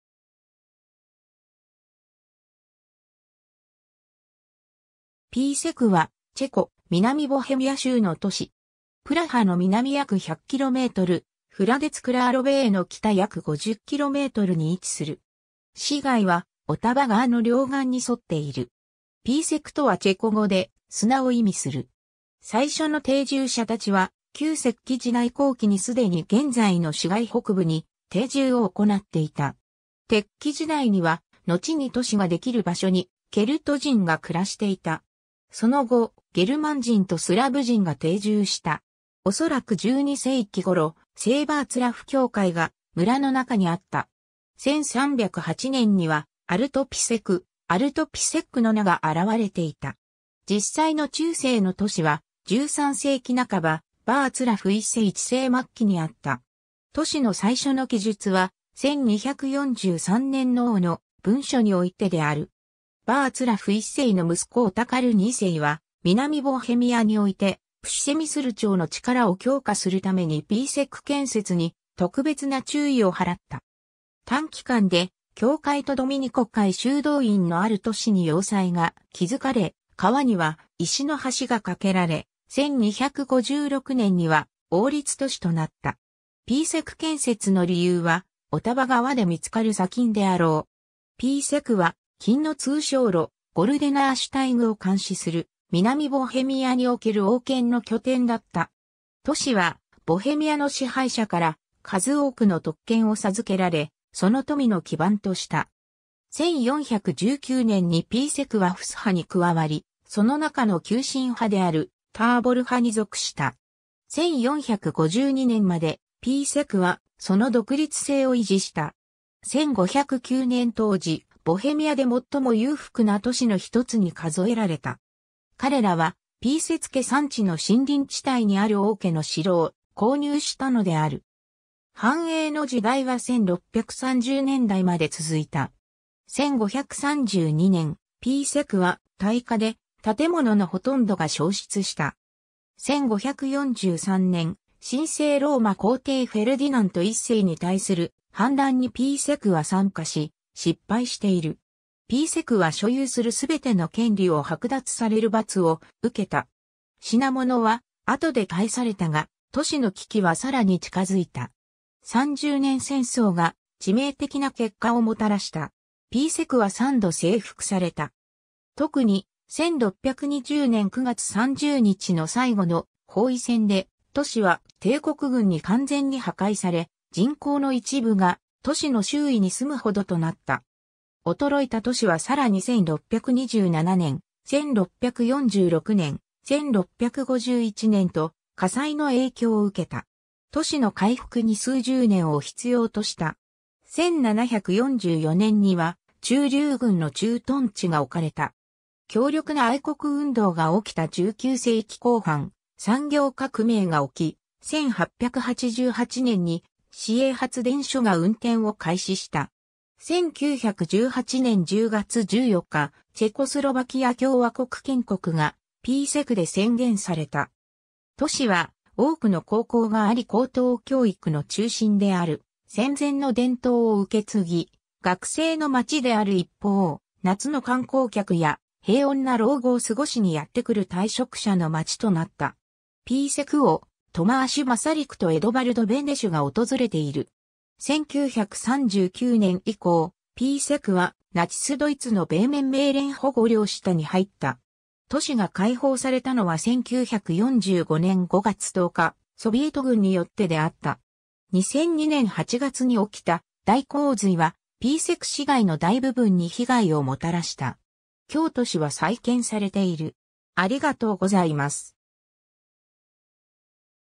ピーセク
ピーセク （チェコ語:Písek、ドイツ語:Pisek）は、チェコ、南ボヘミア州の都市。
 プラハの南約100km、フラデツ・クラーロヴェーの北約50kmに位置する。
 市街はオタヴァ川（ヴルタヴァ川支流）の両岸に沿っている。
ピーセクとはチェコ語で『砂』を意味する。
 最初の定住者たちは、旧石器時代後期に既に現在の市街北部に定住を行っていた。
鉄器時代には、のちに都市ができる場所にケルト人が暮らしていた。
その後、ゲルマン人とスラヴ人が定住した。
 おそらく12世紀頃、聖ヴァーツラフ教会が村の中にあった。
1308年にはアルトピセク、Altpisek（チェコ語でStarý Písek）の名が現れていた。
 実際の中世の都市は、13世紀半ば、ヴァーツラフ1世治世末期にあった。
都市の最初の記述は、1243年の王の文書においてである。
ヴァーツラフ1世の息子オタカル2世は、南ボヘミアにおいてプシェミスル朝の力を強化するためにピーセク建設に特別な注意を払った。
短期間で、教会とドミニコ会修道院のある都市に要塞が築かれ、川には石の橋（現在、チェコ国内最古の石橋）が架けられ、1256年には王立都市となった。
 ピーセク建設の理由は、オタヴァ川で見つかる砂金であろう。
ピーセクは金の通商路ゴルデナー・シュタイグ（de）を監視する、南ボヘミアにおける王権の拠点だった。
都市はボヘミアの支配者から数多くの特権を授けられ、その富の基盤とした。
 1419年にピーセクはフス派に加わり、その中の急進派であるターボル派に属した。
1452年までピーセクはその独立性を維持した。
1509年当時、ボヘミアで最も裕福な都市のひとつに数えられた。
彼らはピーセツケ山地の森林地帯にある王家の城を購入したのである。
繁栄の時代は1630年代まで続いた。
 1532年、ピーセクは大火で建物のほとんどが焼失した。
1543年、神聖ローマ皇帝フェルディナント1世に対する反乱にピーセクは参加し、失敗している。
ピーセクは所有する全ての権利を剥奪される罰を受けた。
品物は後で返されたが、都市の危機はさらに近づいた。
 三十年戦争が致命的な結果をもたらした。
ピーセクは3度征服された。
特に、1620年9月30日の最後の包囲戦で都市は帝国軍に完全に破壊され、人口の一部が都市の周囲に住むほどとなった。
衰えた都市はさらに1627年、1646年、1651年と火災の影響を受けた。
都市の回復に数十年を必要とした。
 1744年には駐留軍の駐屯地がおかれた。
強力な愛国運動が起きた19世紀後半、産業革命がおき、1888年に市営発電所が運転を開始した。
 1918年10月14日、チェコスロバキア共和国建国がピーセクで宣言された（公式宣言は二週間後の10月28日）。
都市は、多くの高校があり高等教育の中心である戦前の伝統を受け継ぎ、学生の町である一方、夏の観光客や、平穏な老後を過ごしにやってくる退職者の町となった。
ピーセクをトマーシュ・マサリクとエドヴァルド・ベネシュが訪れている。
 1939年以降、ピーセクはナチス・ドイツのベーメン・メーレン保護領下に入った。
都市が開放されたのは1945年5月10日、ソビエト軍によってであった。
 2002年8月に起きた大洪水は、ピーセク市街の大部分に被害をもたらした。
今日都市は再建されている。